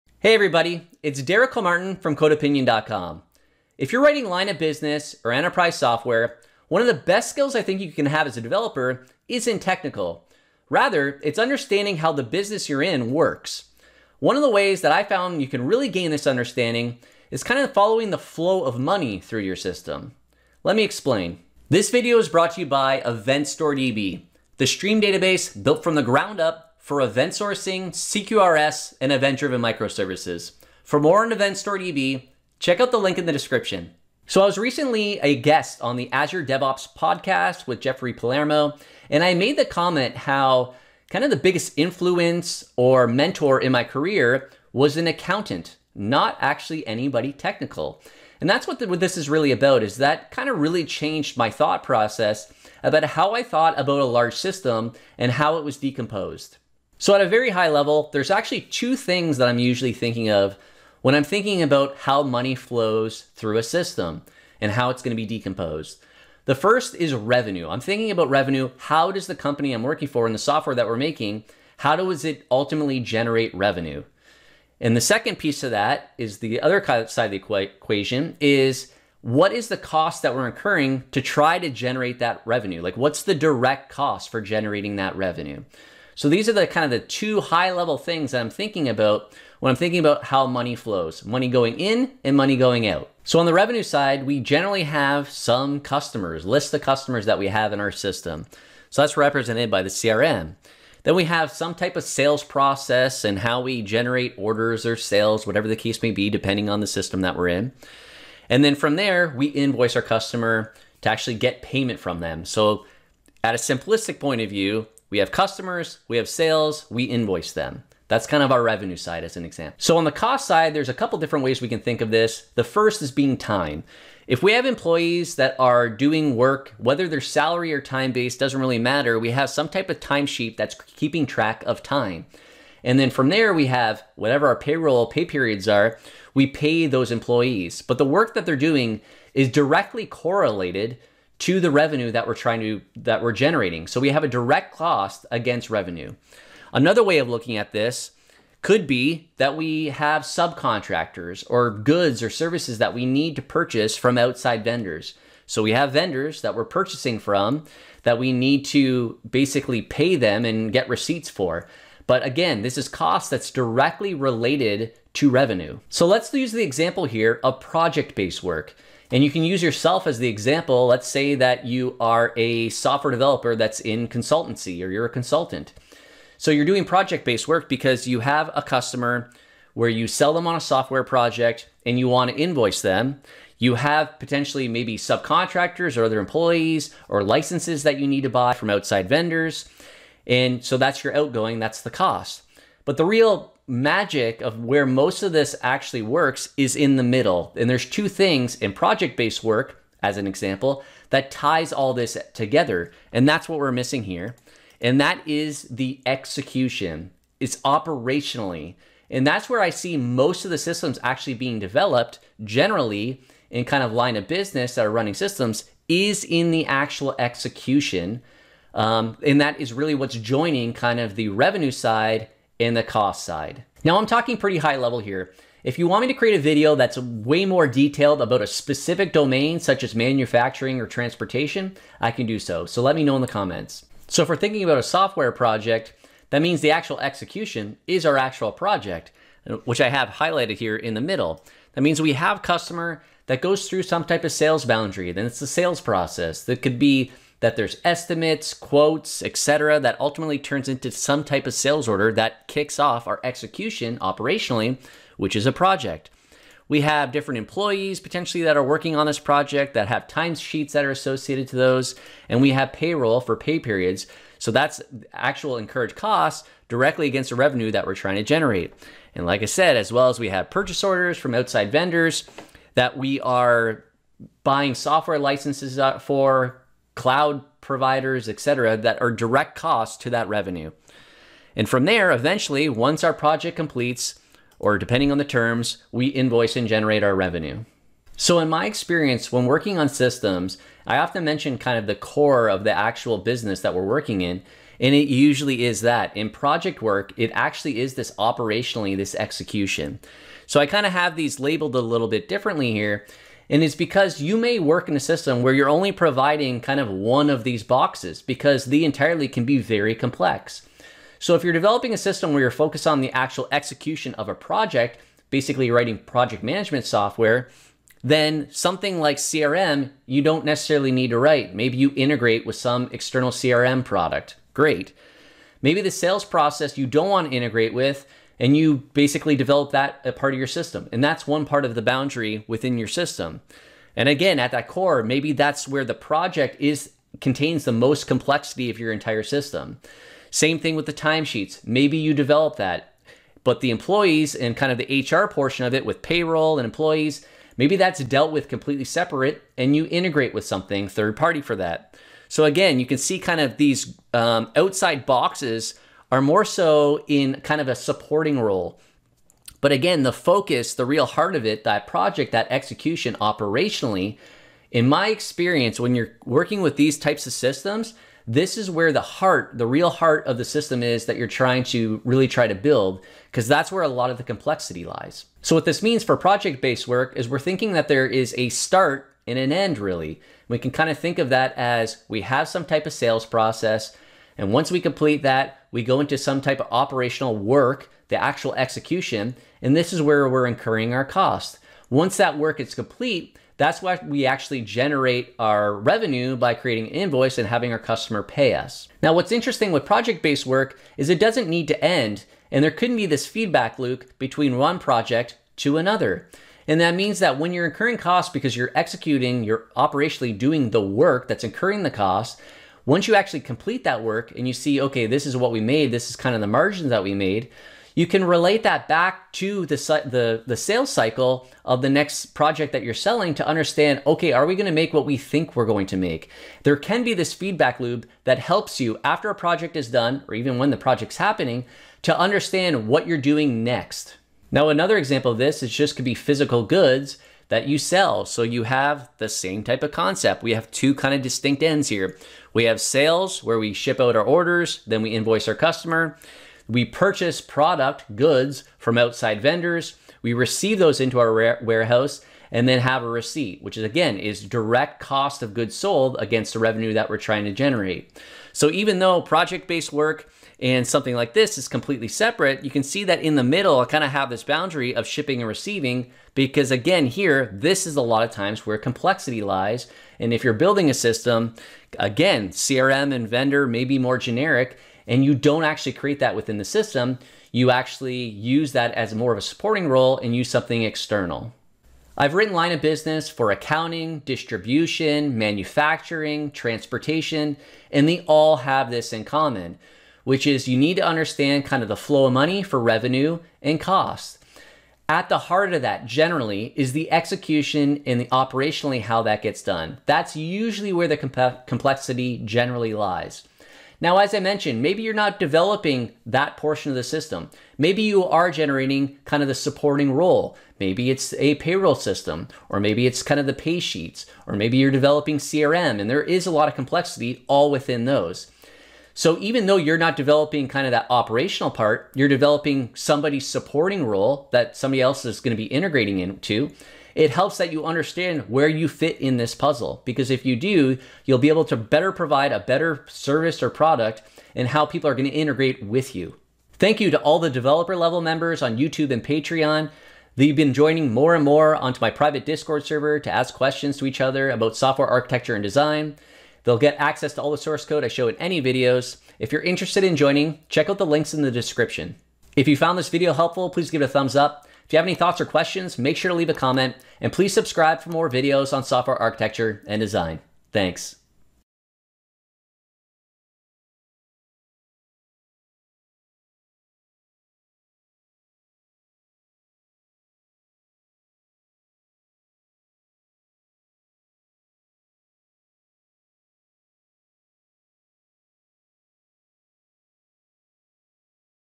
Fintech Mindset to Software Design: How does the Money Flow?
If you're creating line of business or enterprise-type software, I think one of the most valuable skills you can have isn't technical. Rather it's understanding how the business domain you are in works. One way of doing that is following how money flows through a system by having a fintech mindset.

0:00 Intro
0:34 Revenue & Cost
2:46 Operations
3:54 Example: Project
6:15 Lifecycle